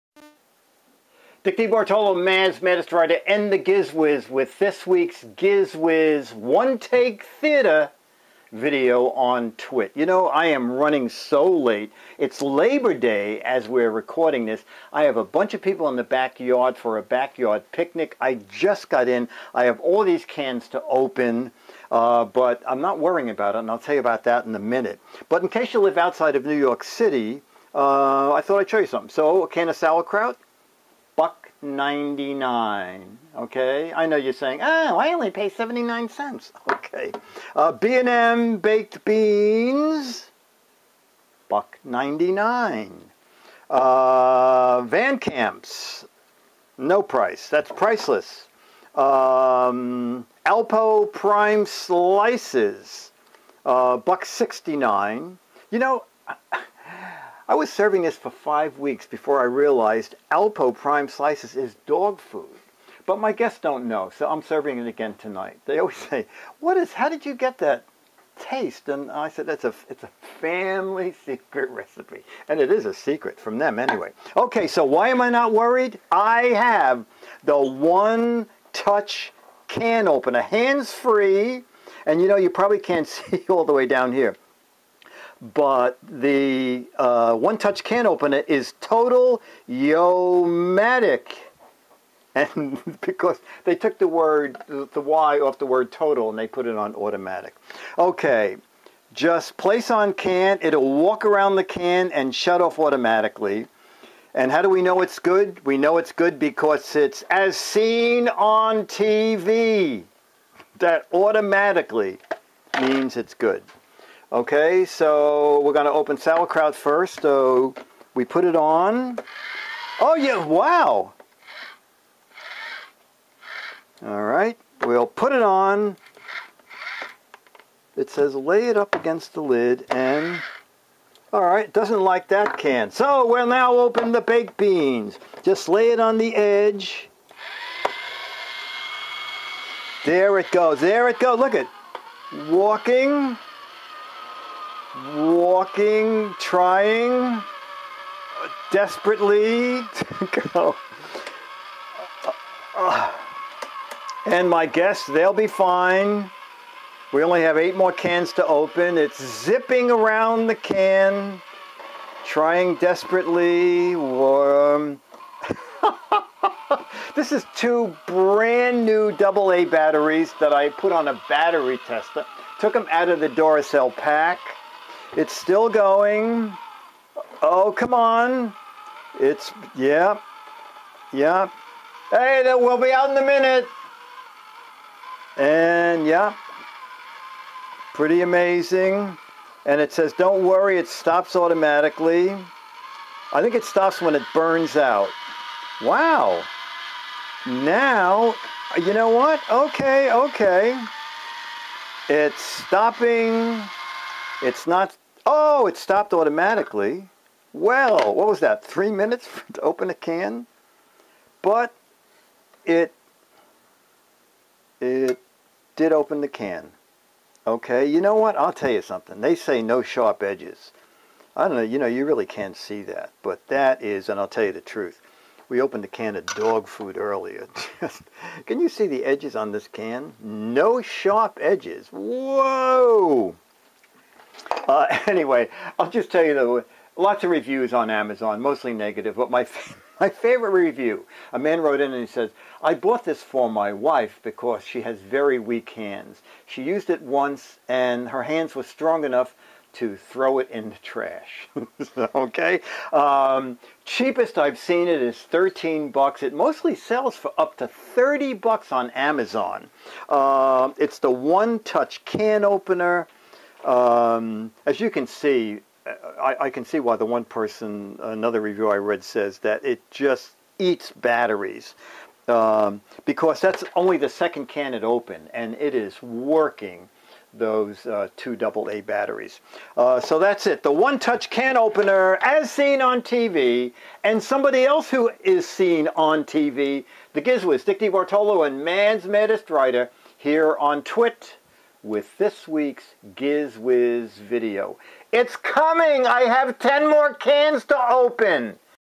The One Touch Automatic Can Opener
This "as seen on TV" can opener is supposed to open can automatically and shut off. It does open some cans, but refuses to open others, at least in my experience.